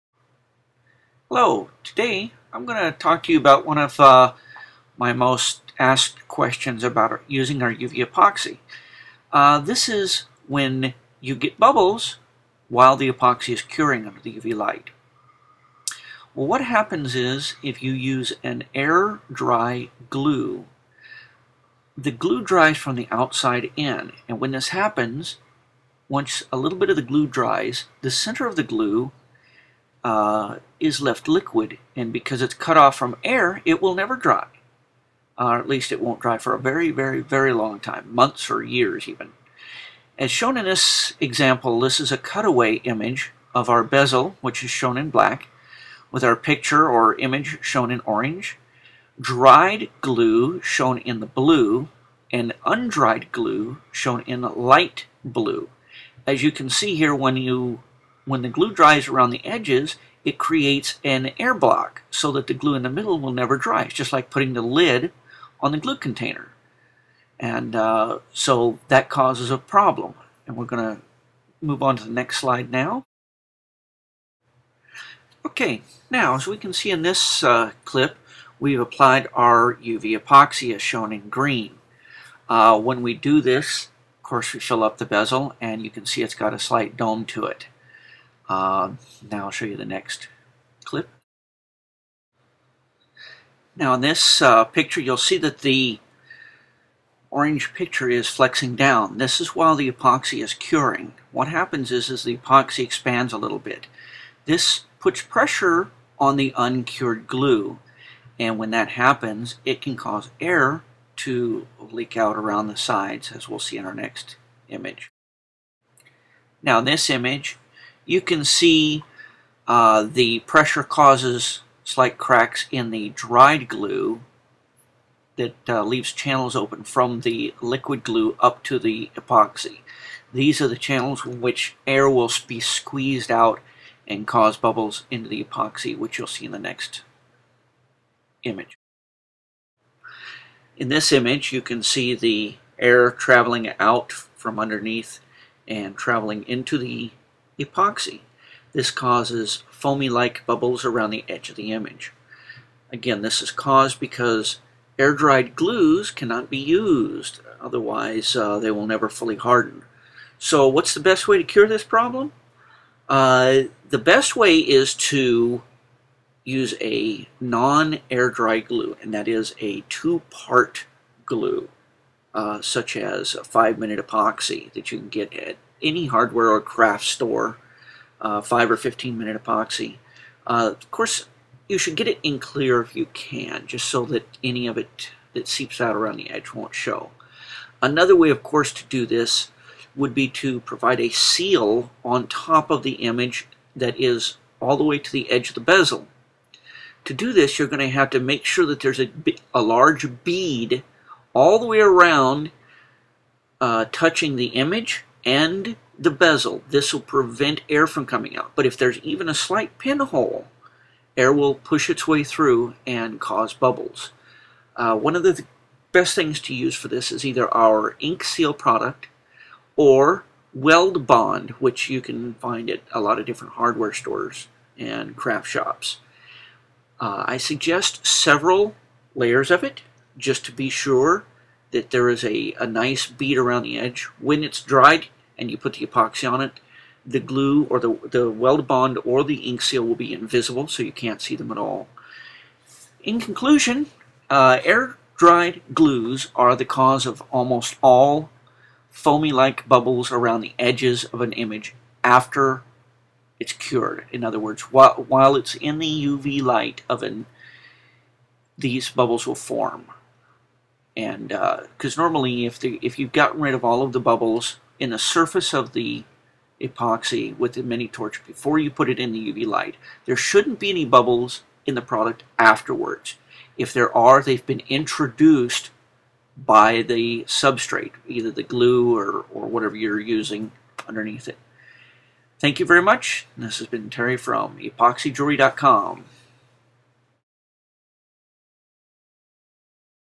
Bubbles Formed in UV epoxy while curing
This slide show will explain what causes the formation of bubbles in our UV epoxy while it's curing. It will also explain how to prevent the problem